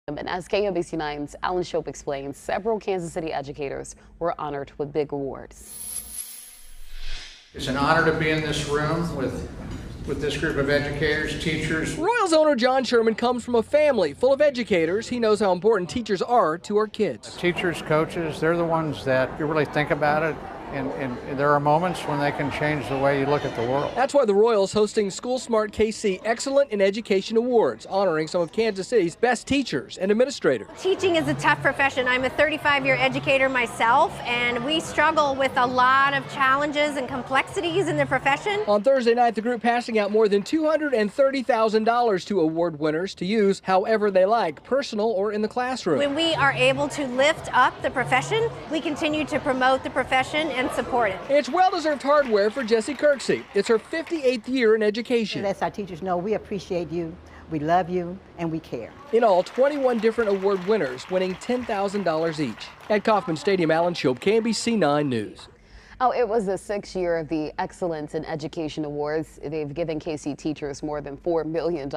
Kansas City Royals honor Kansas City educators with awards and grants
The Kansas City Royals celebrated local educators by hosting the School Smart KC Excellence in Education Awards on Thursday night.